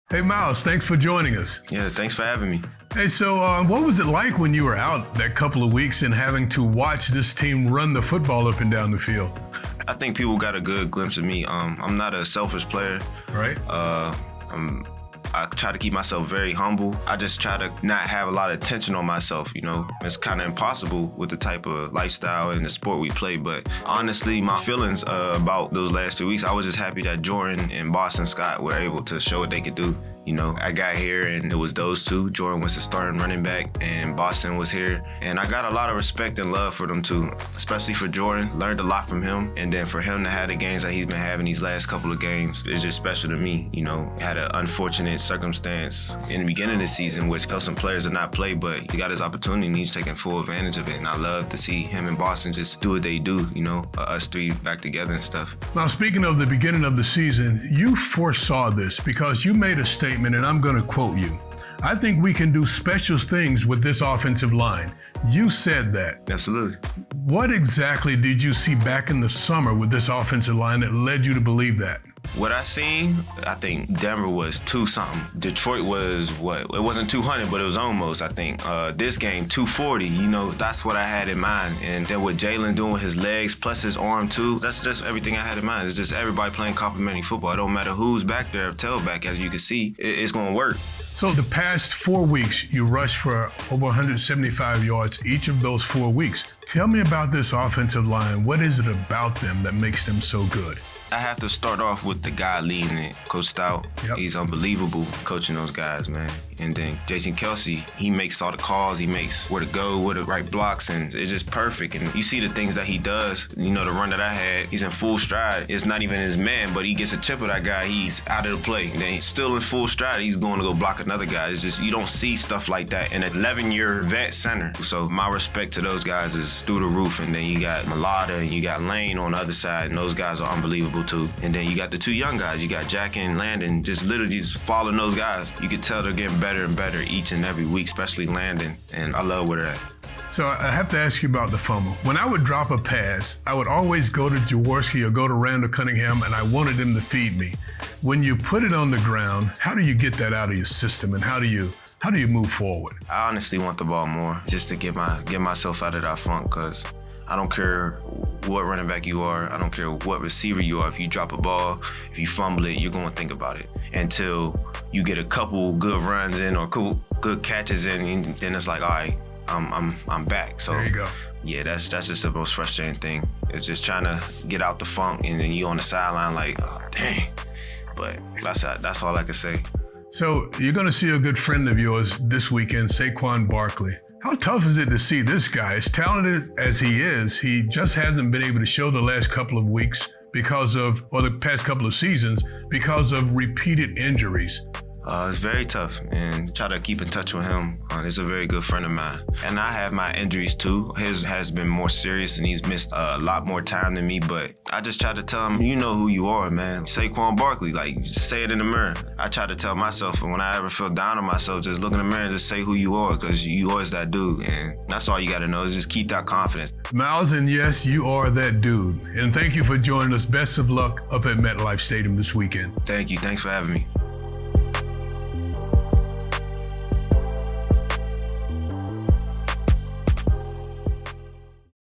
Miles Sanders Discusses the Successful Rushing Attack & More | Eagles One-On-One
Philadelphia Eagles running back Miles Sanders discusses why the rushing attack has been so successful as of late and more in this exclusive interview with Eagles legend Mike Quick.